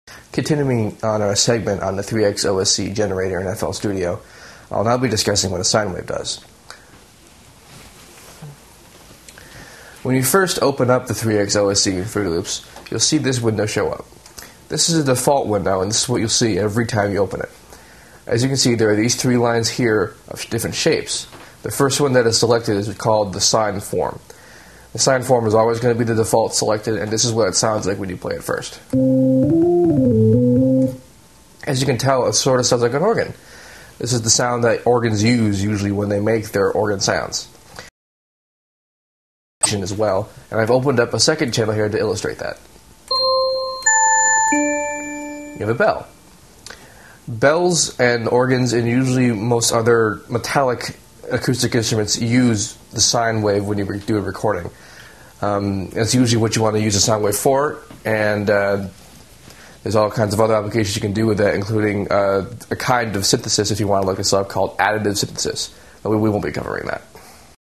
Fruity Loops Studio Tutorial: Sine Wave Generator
Fruity Loops Studio Tutorial: Sine Wave Generator. Part of the series: Fruity Loops Studio: Generators. In the Fruity Loops workstation, sine waves sound like an organ. Learn more about working with sine waves in this free digital audio workstation tutorial on how to use FL (Fruity Loops) Studio from an expert in music recording.